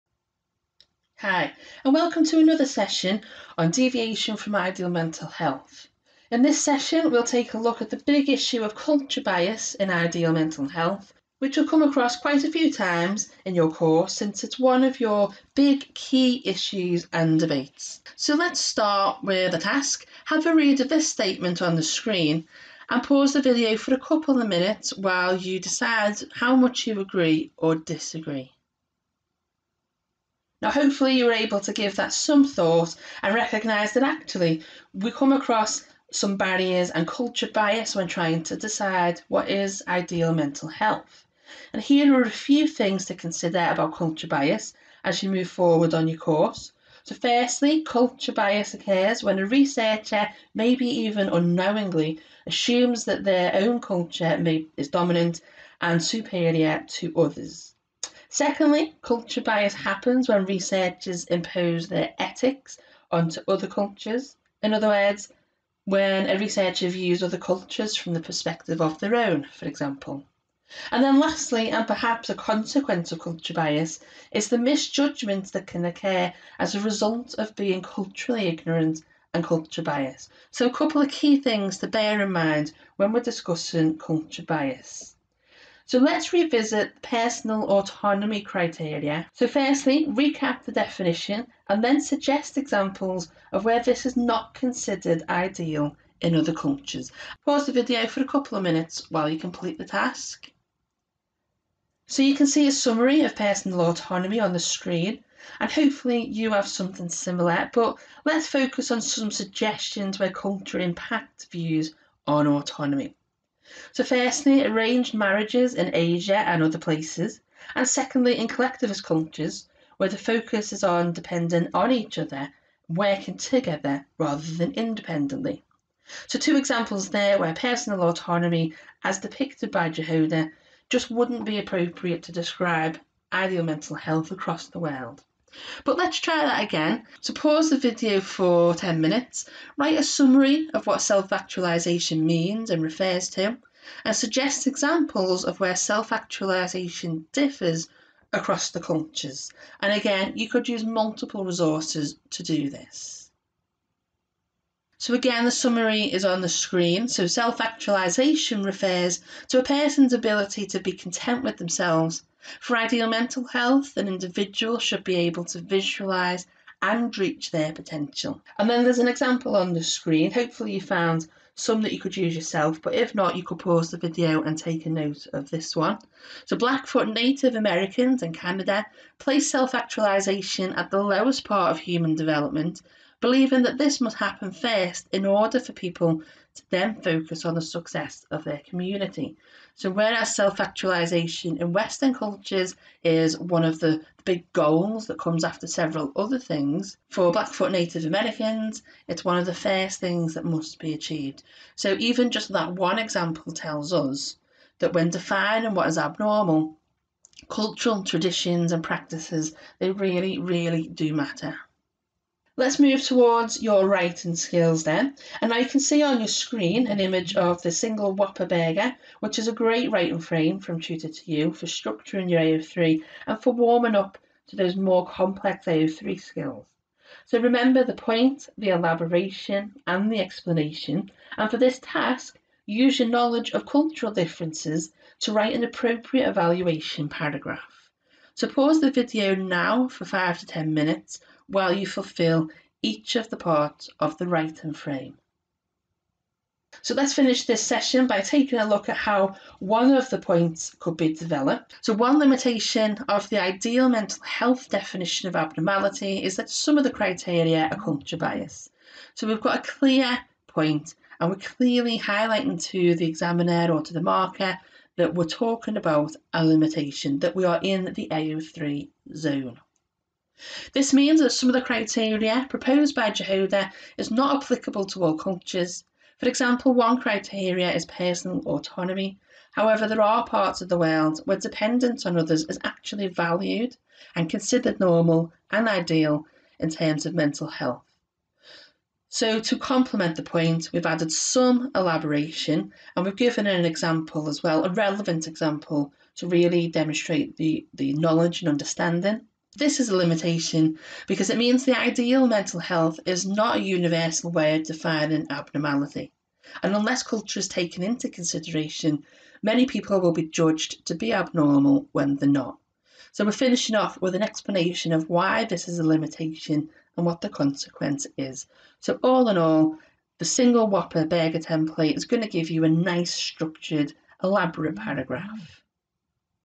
Psychopathology - Deviation from Ideal Mental Health - Culture Bias | AQA A Level Psychology
In this video we explore the role of culture and bias in our perception of ideal mental health.